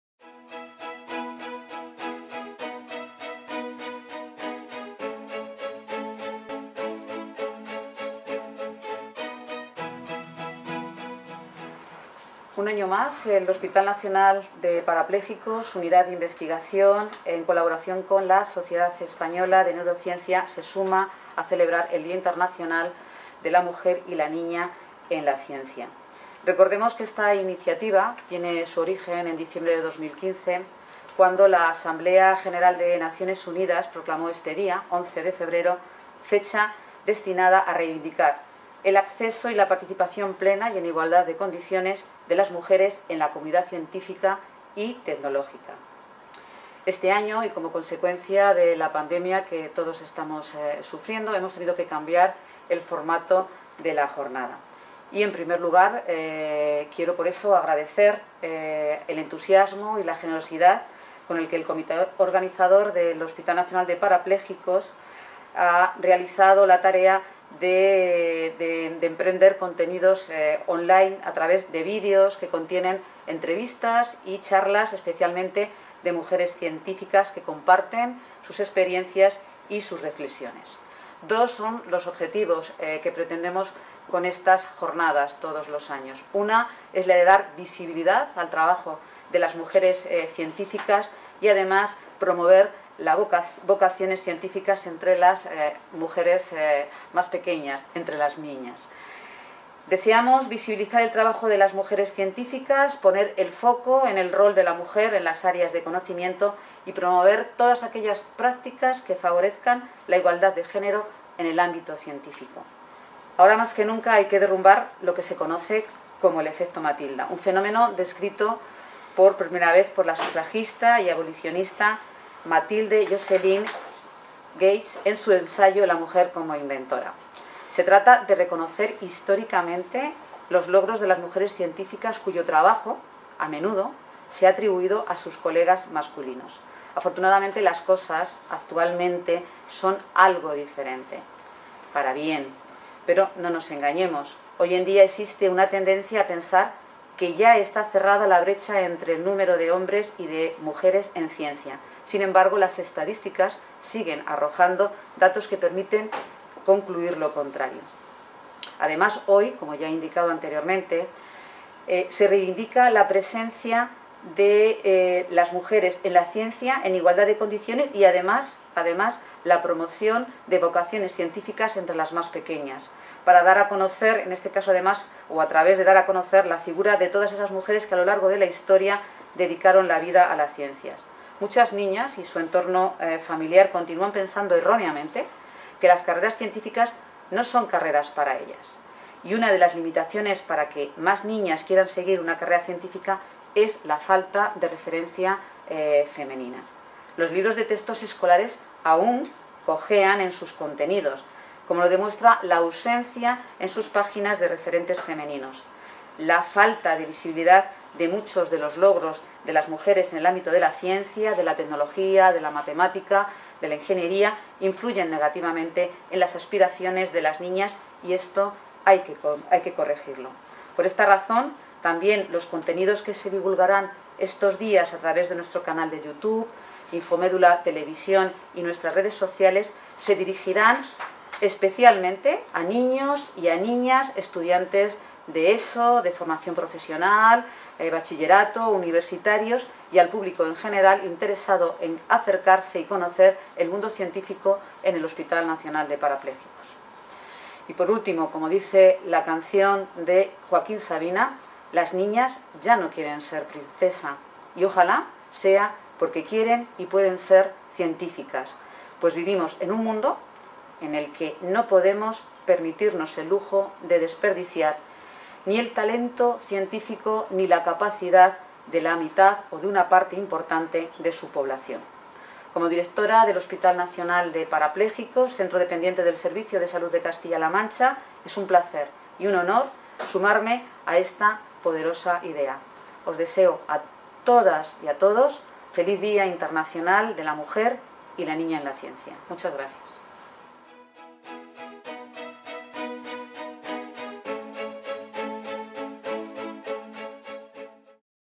Día Internacional de la Mujer y la Niña en la Ciencia 2021.Sagrario de la Azuela, directora HNP.
ACTIVIDAD11f Presentación de Sagrario de la Azuela, directora del Hospital Nacional de Parapléjicos del Día Internacional de la Mujer y la Niña en la Ciencia, en un año , 2021, condicionado por la COVID en el que desarrollamos las actividades y eventos on line.